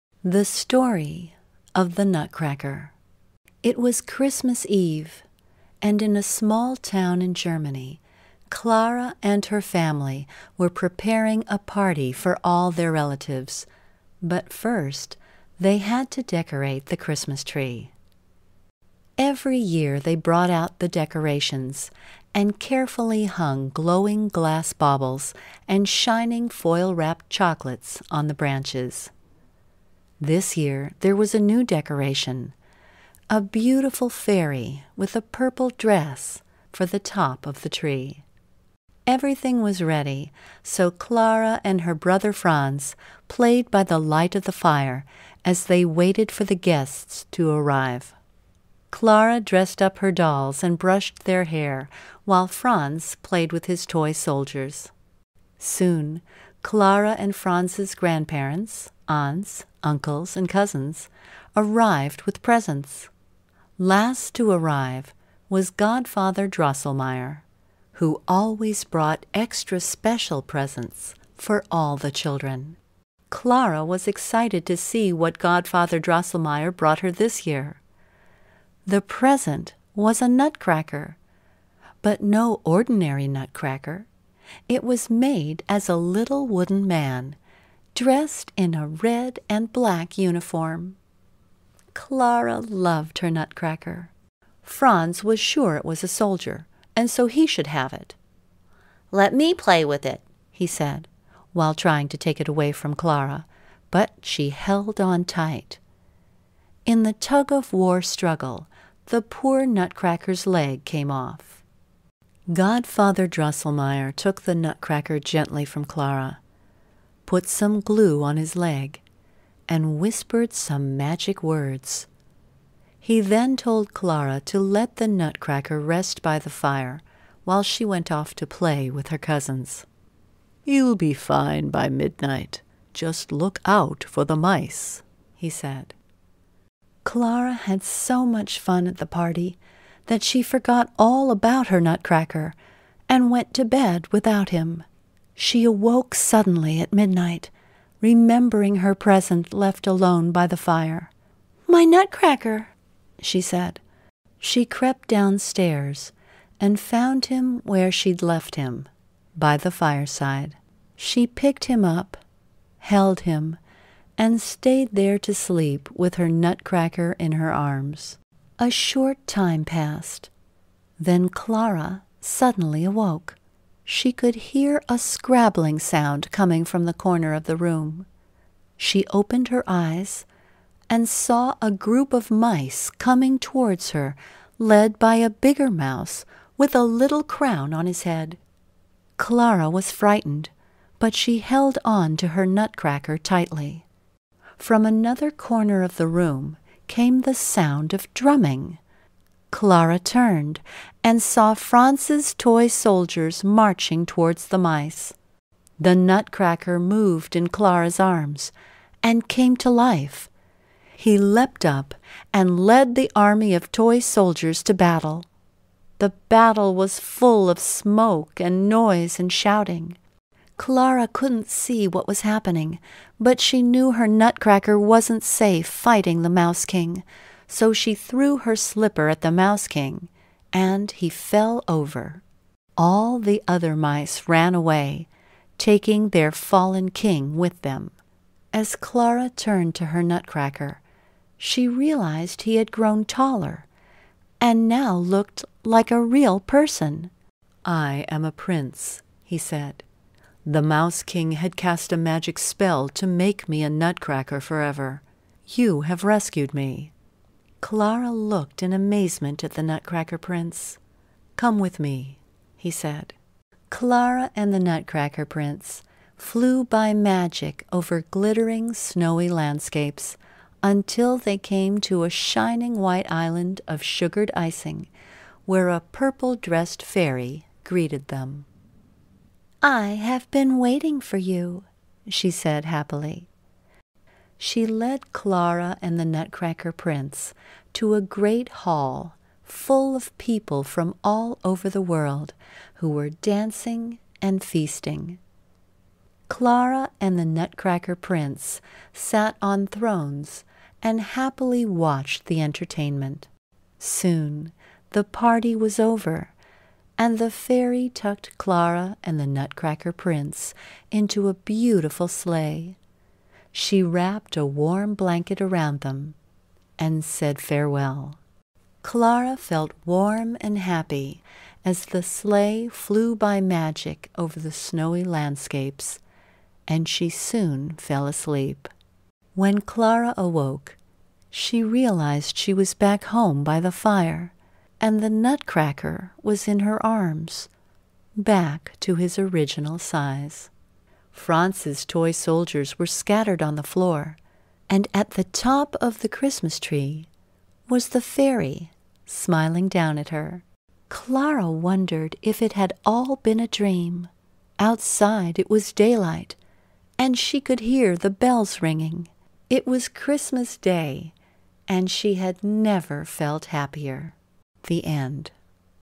Read Along! - The Nutcracker
It's Christmas Eve, and Clara and her brother Franz are playing when her family begins to arrive for the party, carrying gifts for the children.
Her godfather, Drosselmeyer, always brings something special, and this year is no exception: a lovely Nutcracker!